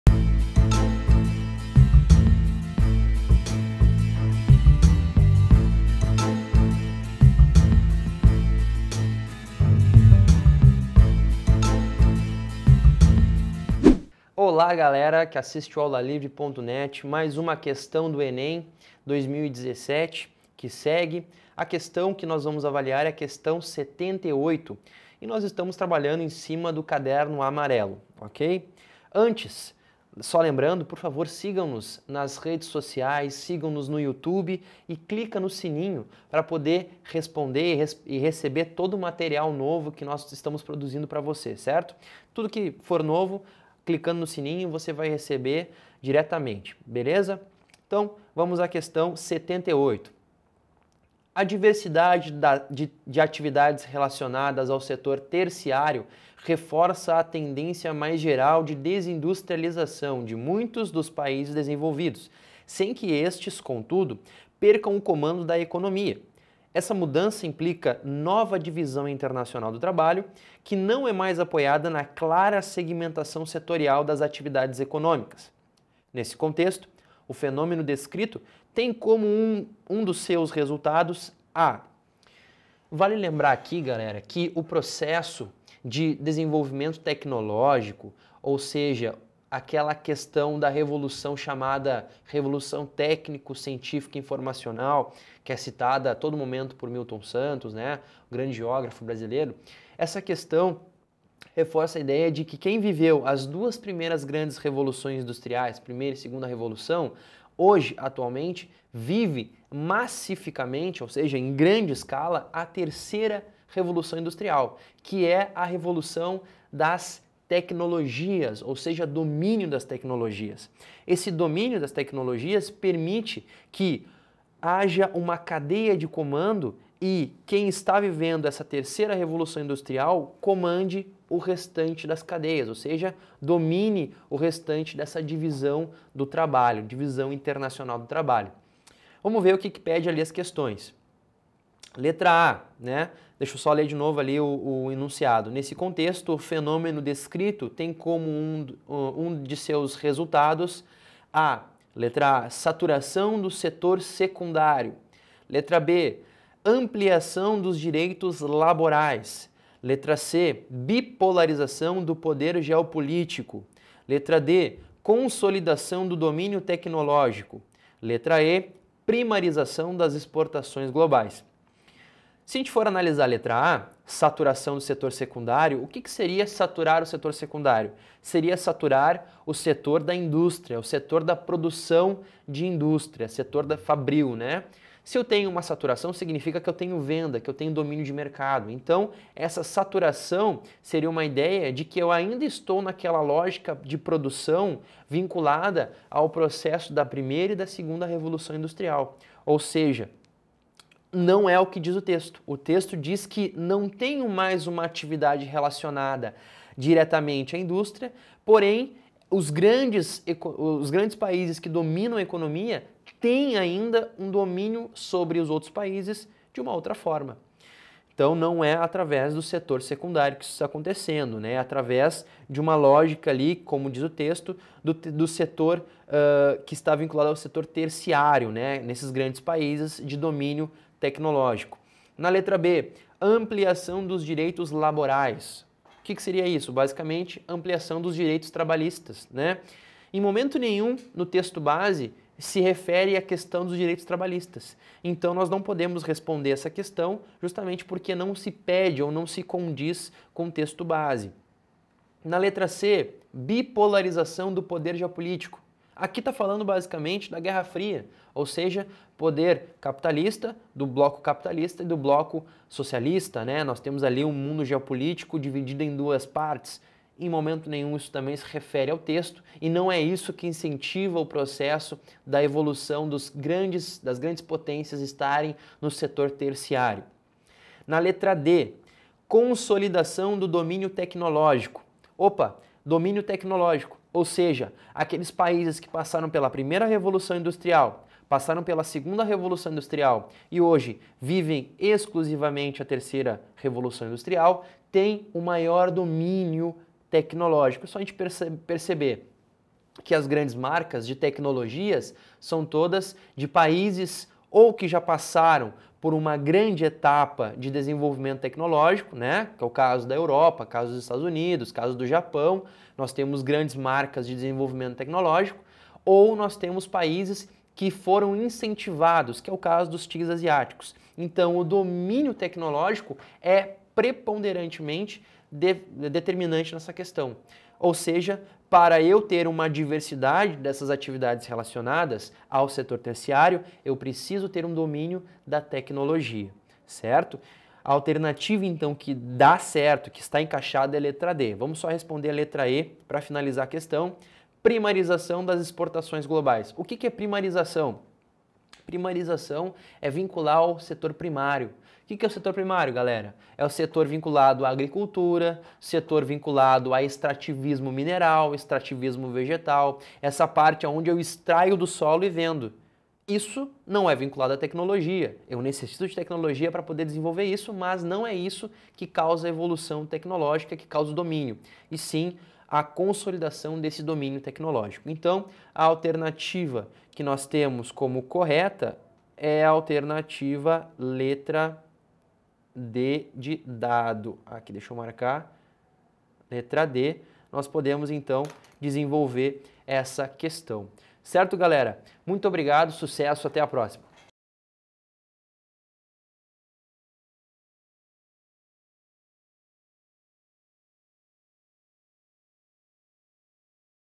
Enem 2017 - Ciências Humanas | Questão 78 (Caderno Amarelo)
Na questão 78 de geografia do caderno amarelo de ciências humanas, é preciso ter conhecimento sobre geoeconomia e os processos ligados à industrialização para responder a questão.

Gostou desse vídeo? Então acesse nosso site e fique ainda mais preparado com nosso curso completo